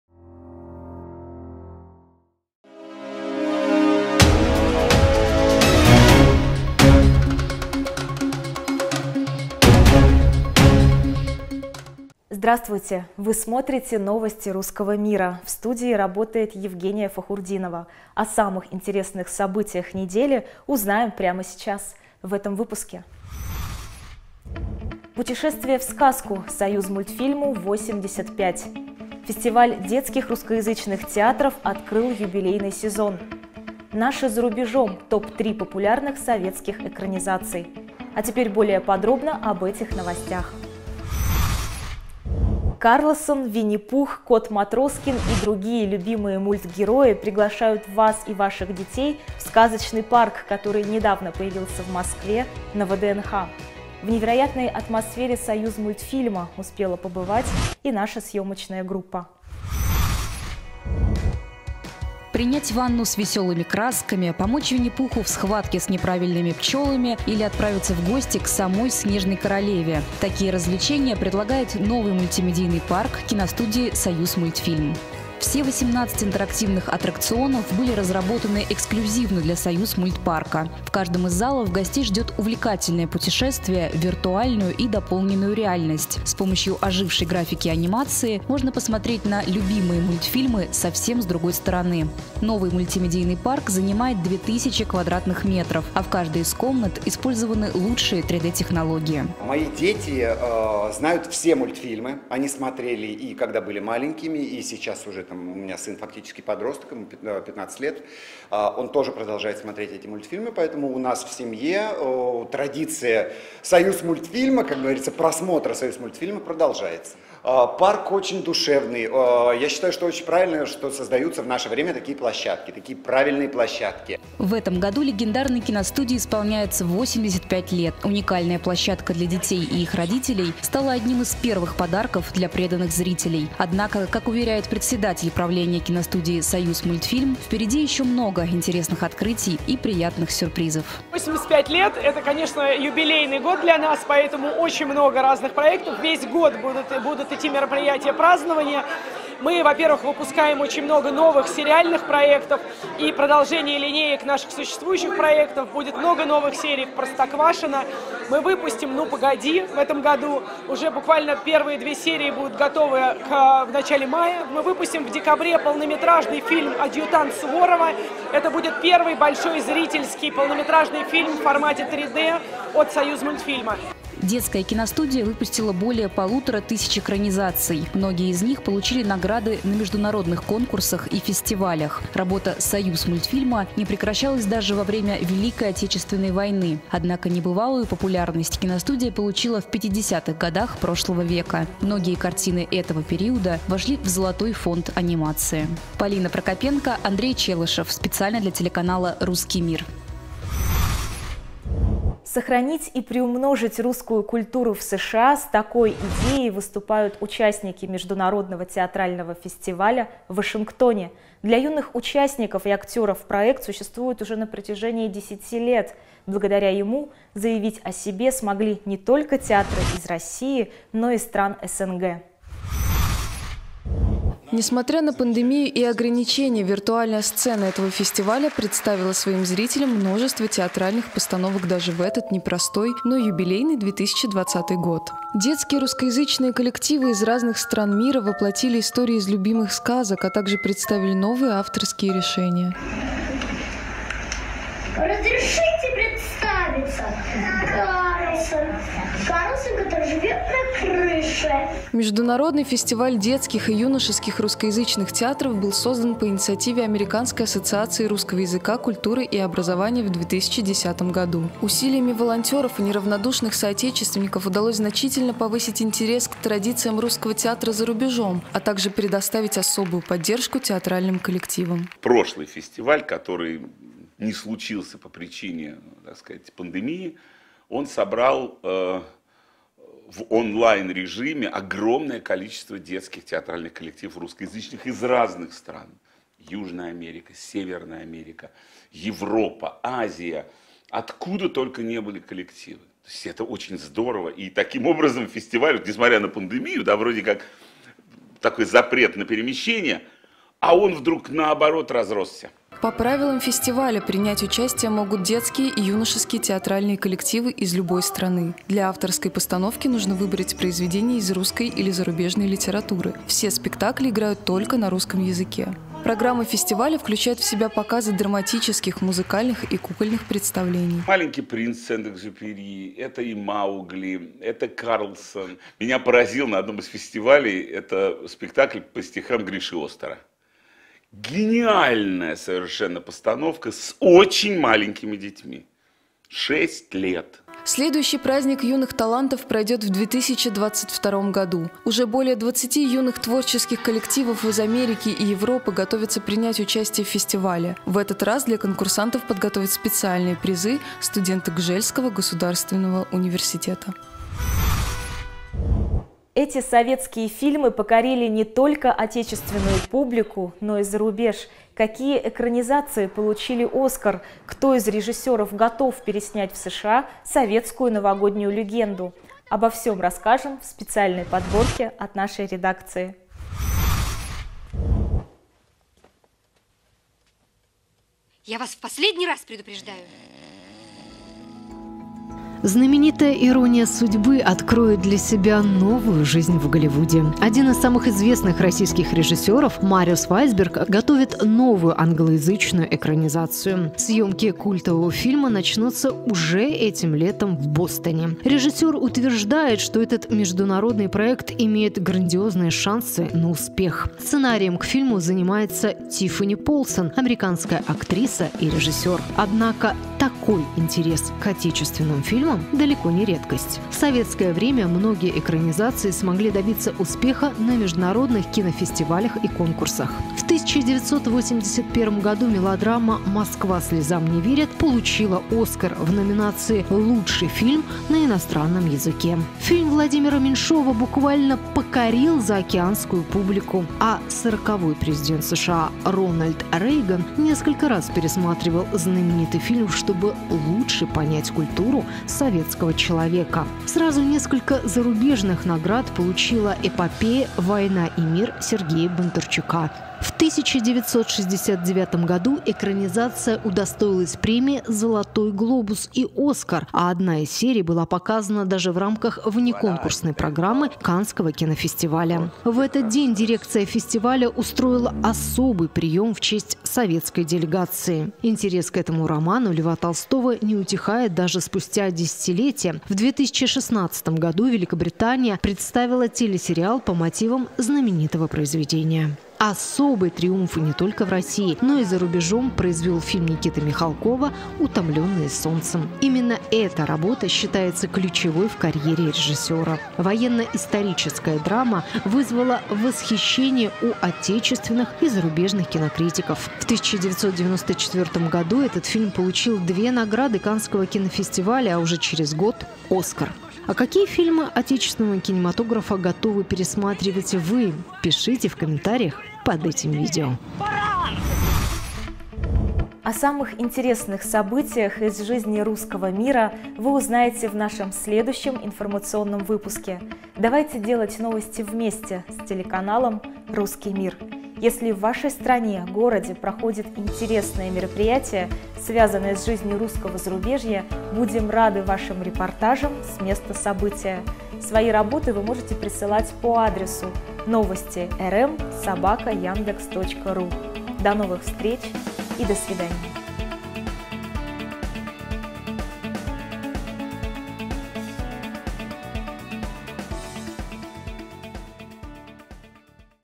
Новости Русского мира. Союзмультфильму 85!
В этом выпуске:

— Путешествие в сказку: киностудии «Союзмультфильм» 85 лет;

— Фестиваль детских русскоязычных театров открыл юбилейный сезон;

— Наши за рубежом: какие экранизации получили золотой «Оскар»? Кто из режиссёров готов переснять в США советскую новогоднюю легенду? Обо всём расскажем в специальной подборке от нашей редакции.